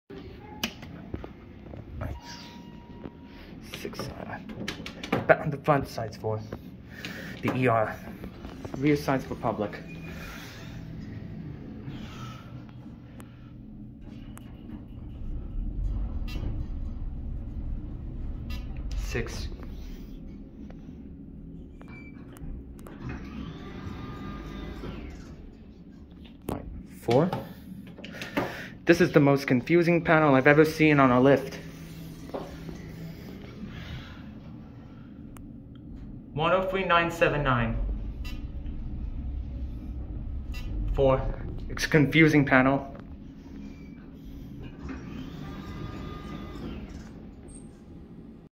CONFUSING 2009 ThyssenKrupp Traction Birch Elevator | Highline Medical Center | Burien WA
Now the Saint Anne Hospital, This has THE MOST confusing lift COP I have ever come across!

Installed 2009